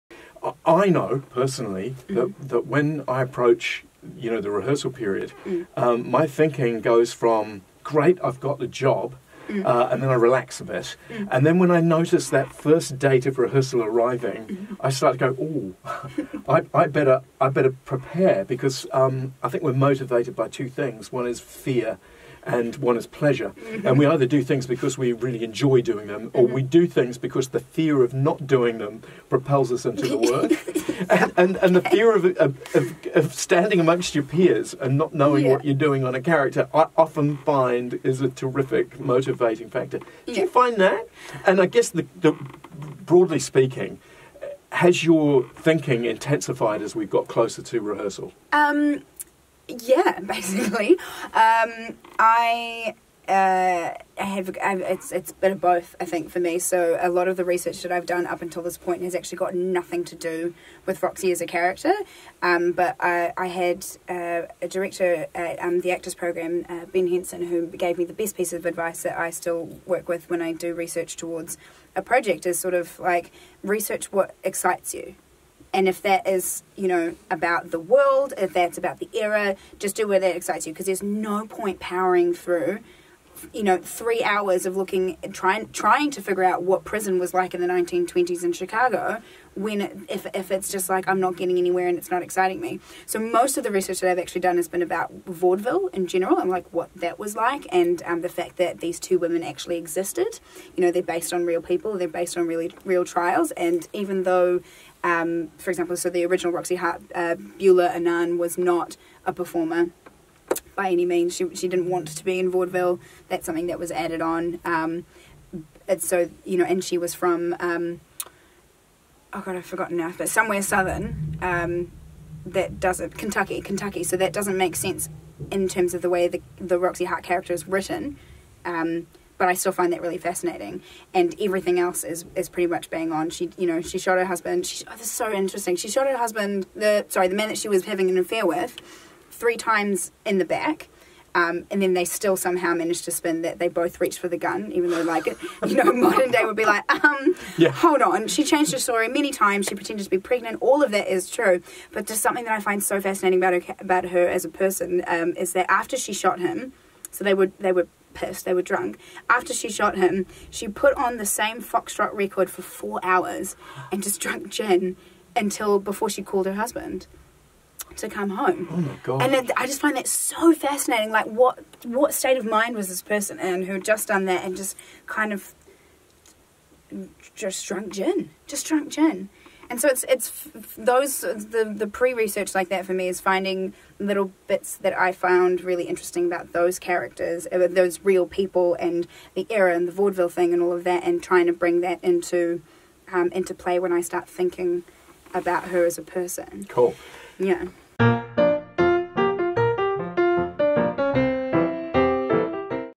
Nomi Cohen chats to Ross Gumbley - Research
After their first conversation about auditions, finding the character and Chicago, Ross and Nomi sat down once again to discuss what kind of research Nomi does on her character and the show before rehearsals begin.